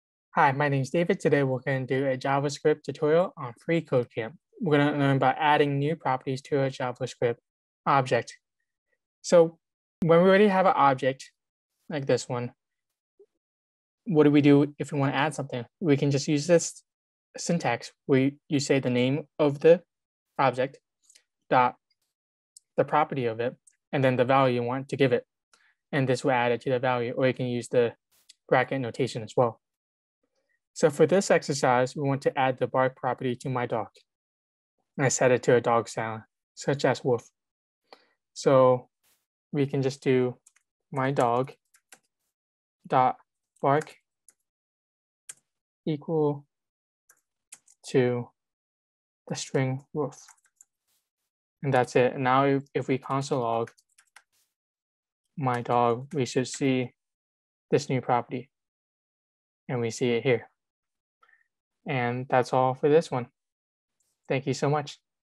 Add New Properties to a JavaScript Object (Basic JavaScript) freeCodeCamp tutorial
Certification: JavaScript Algorithms and Data Structures
Course: Basic JavaScript
Lesson: Add New Properties to a JavaScript Object
freeCodeCamp tutorial

In this lesson, we learn how to add new properties to an object.